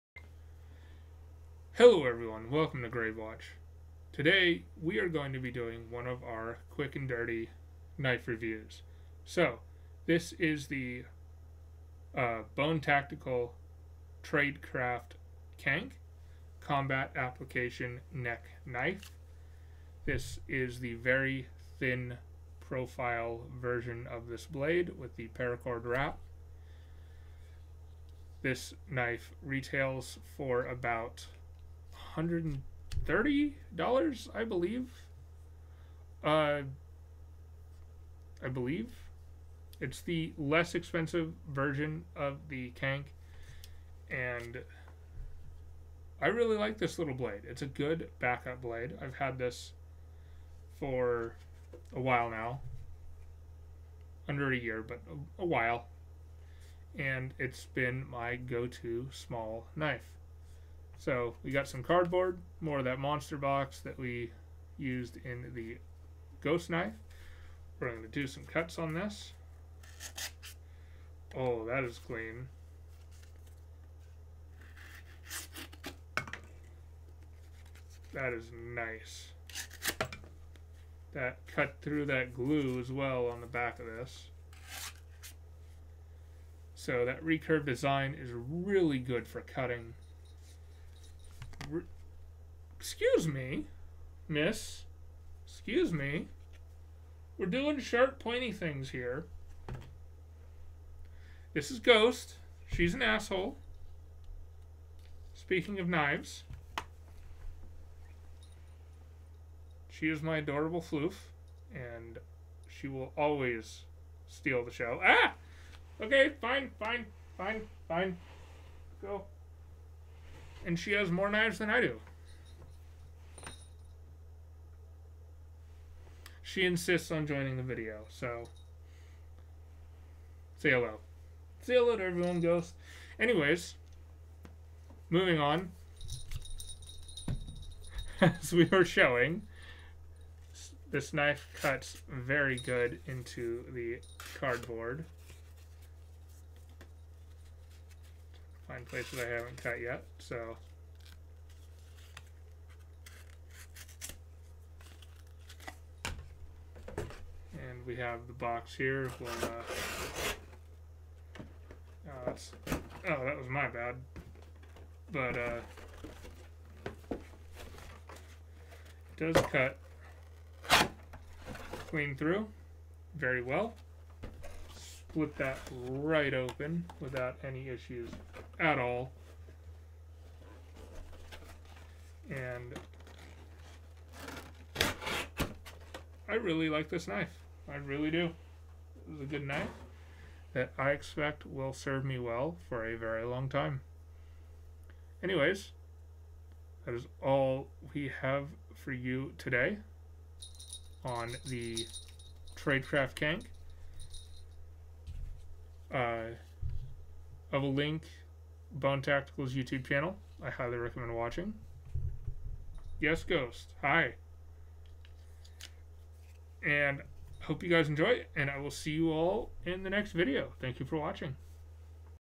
Tradecraft CANK Dirty Review
Quick overview of the Bone Tactical Tradecraft CANK

Welcome to Project Gravewatch, tactical, gaming, reviews and more! Join us as we do lots of dumb stuff!